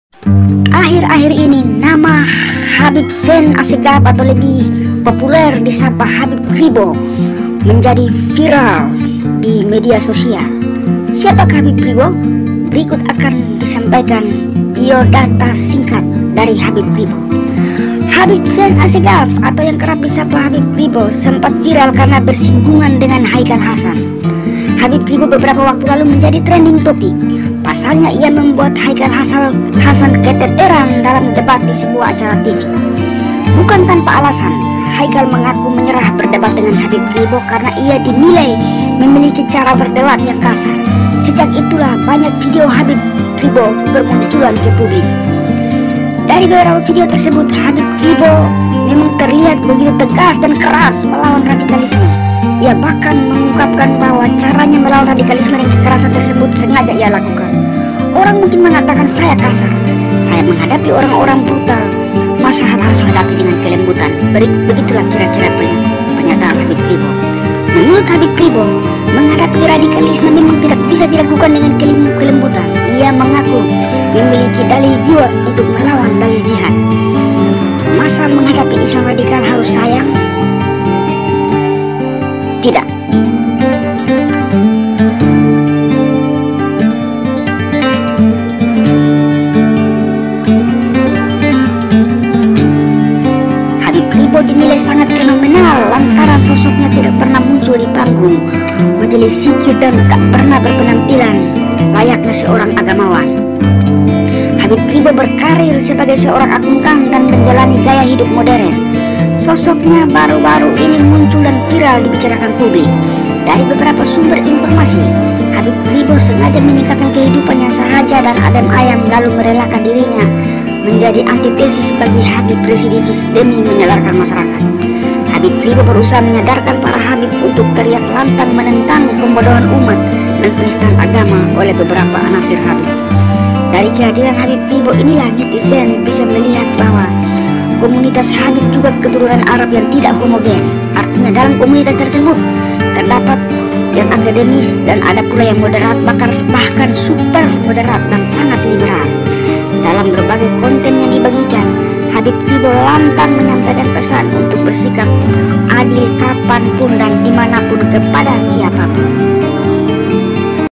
sekilas mengenal habib kribo (Zein Assegaf)
sekilas mengenal habib kribo (Zein Assegaf). Nama asli Habib Kribo adalah Zein Assegaf. Ia dikenal dengan nama Habib Kribo lantaran rambutnya yang kribo. nama habib kribo akhir-akhir menjadi topik perhatian nitzen lantaran gaya habib kribo yang blak-blakakn melawan radikalisme dan habib krobo juga menyadarkan banyak khalayak bahwa seorang habib di arab tidak selalu harus indentik dengan atribut keagamaan. bagaiman sosok habib kribo dengan fenomenanya akan di bahas sera ringkas dalam video ini.

semoga video ini bermanfaat bagi kiat semua..salam cinta NKRI